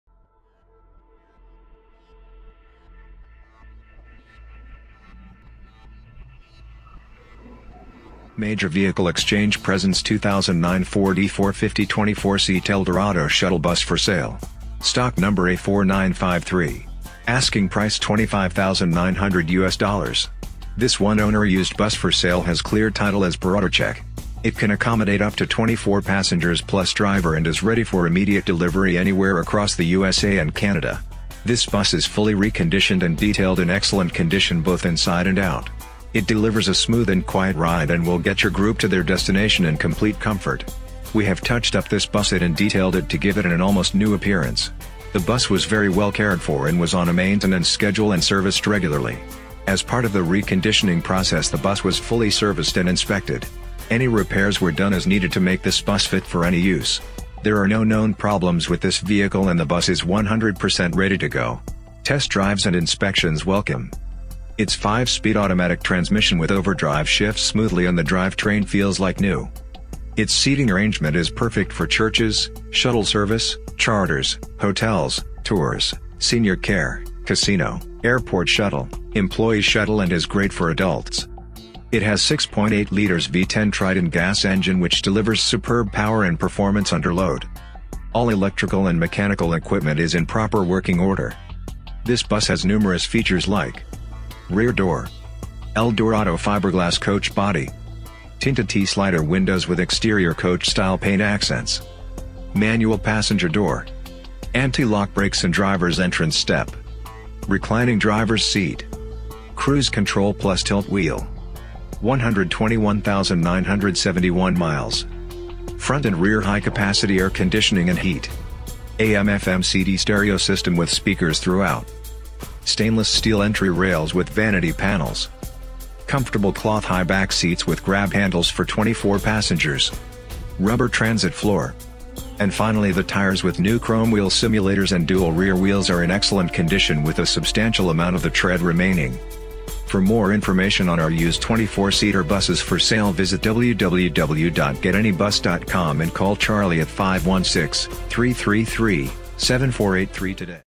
Used 24 seat buses For Sale | 2009 Ford E450 24 Seat Eldorado Shuttle Bus
2009 Ford E450 24 Seat Eldorado Shuttle Bus
For Senior Tour Charters Student Church Hotel Transport
All Fiberglass Eldorado Aerotech Shuttle Bus in Excellent Condition

Condition Used Clear Title
Miles 121,971
Interior
Stock # A4953
VIN # 1FDFE45S69DA61741
Some of our used vehicles may be subject to unrepaired safety recalls. Check for a vehicle’s unrepaired recalls by VIN
Vehicle Overview
FORD  E-450 ELDORADO SHUTTLE BUS - 24 PASSENGERS PLUS DRIVER

FORD 6.8L V-10 GAS ENGINE - FIVE SPEED AUTOMATIC TRANSMISSION WITH OVERDRIVE - EXCELLENT AIR CONDITIONING/HEAT FRONT & REAR - ANTI-LOCK BRAKES.

PERFECT FOR CHURCHES, SHUTTLE SERVICE, CHARTERS, HOTELS, TOURS, SENIOR CARE, CASINO, AIRPORT SHUTTLE & EMPLOYEE SHUTTLE - GREAT FOR ADULTS.

THIS BUS IS FULLY RECONDITIONED & DETAILED IN EXCELLENT CONDITION BOTH INSIDE AND OUT! THE INTERIOR IS MINT.

MECHANICALLY IT'S READY TO HIT THE ROAD! AND HAS JUST BEEN SERVICED.
COMPLEMENTARY AIRPORT PICKUP OR DELIVERY ARRANGED ANYWHERE IN THE USA AND CANADA.

Major Vehicle Exchange presents this Eldorado Aerotech 2009 E-450 Super Duty StarTrans 24 passenger plus driver shuttle bus. Equipped with a durable and reliable Ford Triton V-10 6.8L gas engine and a 5 speed automatic transmission with overdrive. Just 121k miles. It delivers a smooth and quiet ride and will get your group to their destination in complete comfort thanks to dual A/C and heat & high-back cloth seating.
We have touched up this bus it & detailed it to give it an an almost new appearance. The bus was very well cared for and was on a maintenance schedule and serviced regularly. As part of the reconditioning process the bus was fully serviced and inspected. Any repairs were done as needed to make this bus fit for any use. All electrical and mechanical equipment is in proper working order. There are no known problems with this vehicle and the bus is 100% ready to go. Inspections welcome!

FEATURES & SPECIFICATIONS:
FORD: E-450 Super Duty 176" Wheelbase Dual Rear Wheels - Eldorado all Fiberglass Coach Body - Ford Gas 6.8L V-10 Triton - 5 Speed Automatic With Overdrive - Manual Passenger Door -  Drivers Entrance Step - Cruise Control - Tilt Wheel - Overhead Lighthing - Comfortable Cloth High-Back Seats w/ Grab Handles - Tinted T-Slider Windows with Exterior Coach Style Paint Accents - Overhead Hand Rails - Air Conditioning and Heat Front and Rear - AM/FM/CD Stereo - New Chrome Wheel Simulators - 24 Passengers plus Reclining Drivers Seat - Rear Door - DOT Equipment.

Used 24 seat buses For Sale, 2009 Ford E450 24 Seat Eldorado Shuttle Bus, KGN7